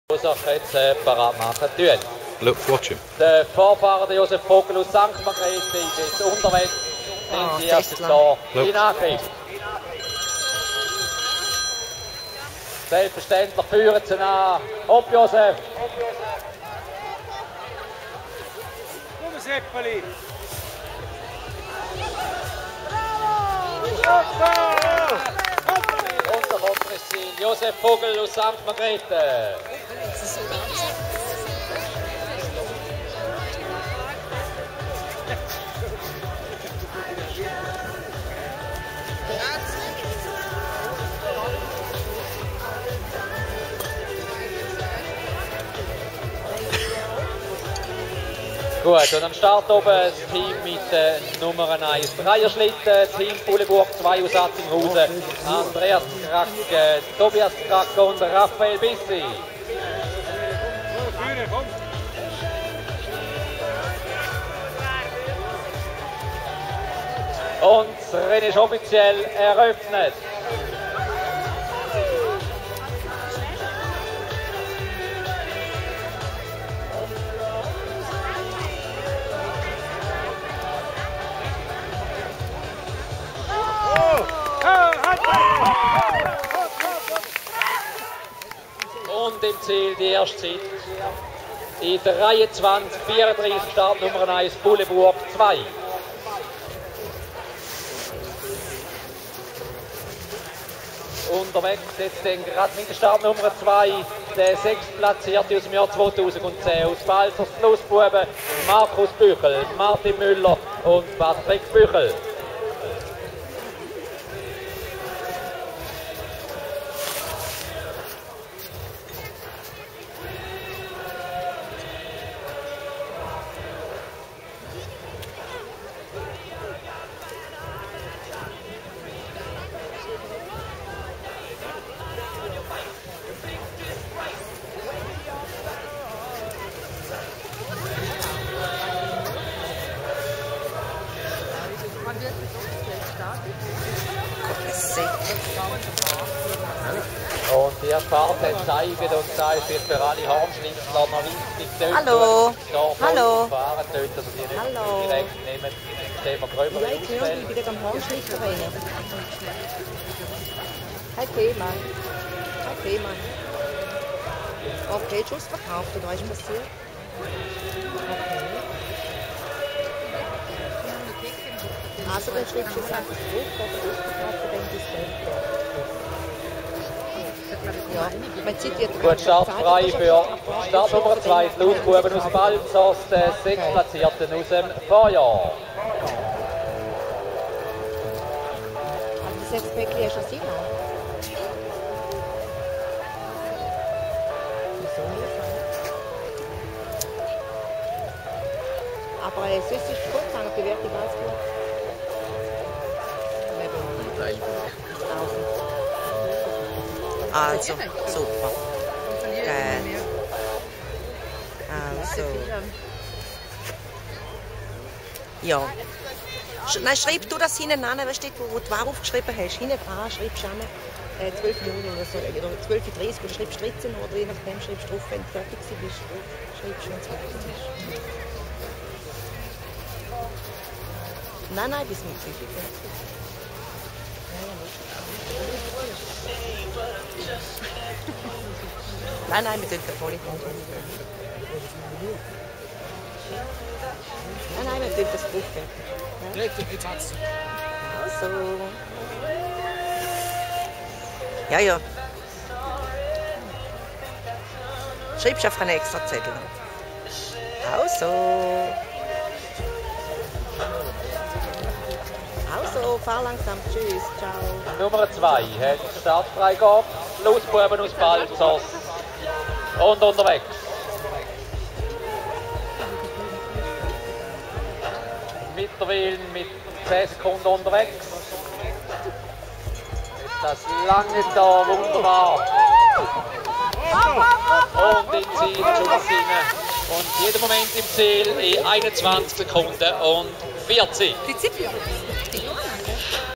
Hornslchlittenrennen Alt st Johann 2011
2:54Add toAdded to queue video lang: en
Translate
View original
(Translation disabled)Hornschlittenrennen  Alt St Johann Switzerland
Sledge Racing in Switzerland
19/02/2011